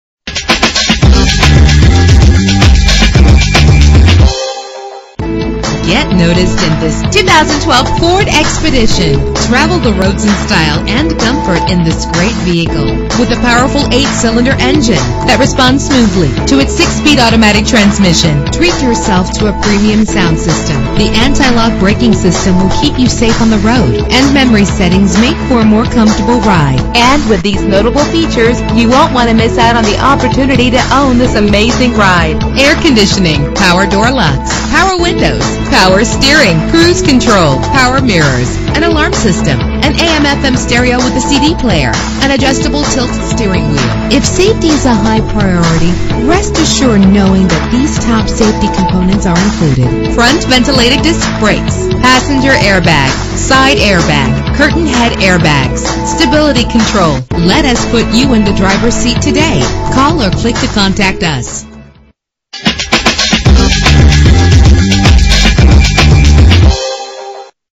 2012 Ford Expedition - Tampa FL
Year: 2012
Make: Ford
Model: Expedition
Trim: Limited
Engine: 5.4 liter 8 cylinder 24 valve
Transmission: 6-Speed Automatic
Color: White
Mileage: 2
Address:
7201 West Linebaugh Avenue